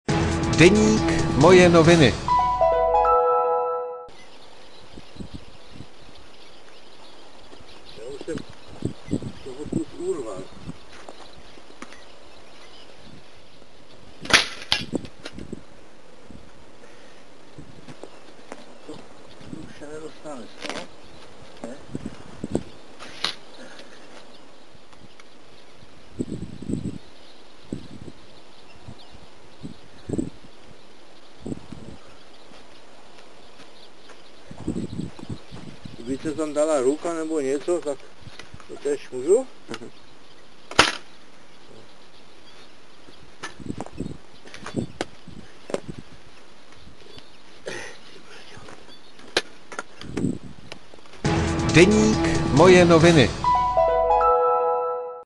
Myslivec Jan Adamek ukazuje pytlácké pasti
Havířovští myslivci našli v lesích pytlácké nástrahy. Jan Adamek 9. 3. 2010 ukazuje, jak pasti fungují. Více na